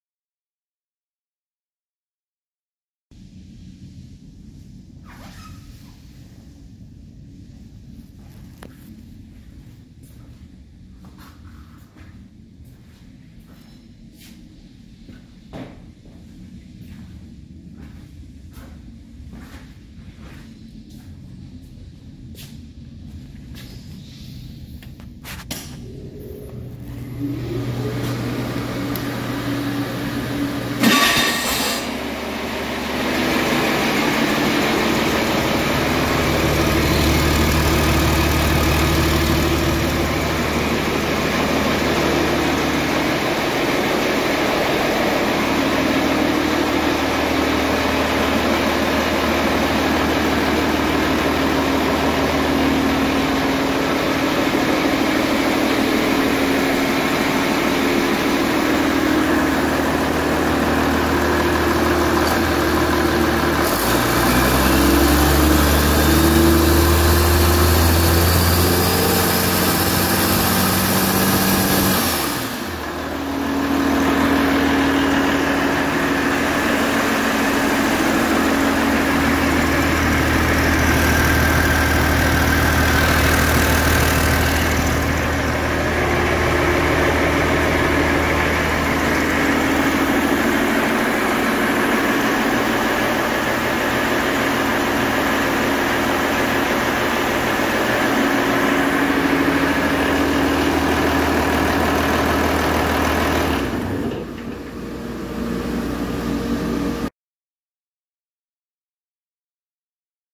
Rebuilt 1983 -1987 International DT466C Engine Test Run 500-16228
Good Rebuilt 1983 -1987 International DT466C being test run. Engine Serial Number: 468TM3U454928 with 210 hp. Engine was removed from a IHC S-line and runs excellent. Our Stock Number is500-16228.

Frontier Truck Parts has one of the largest inventories of Engines & Engine Parts in the Midwest.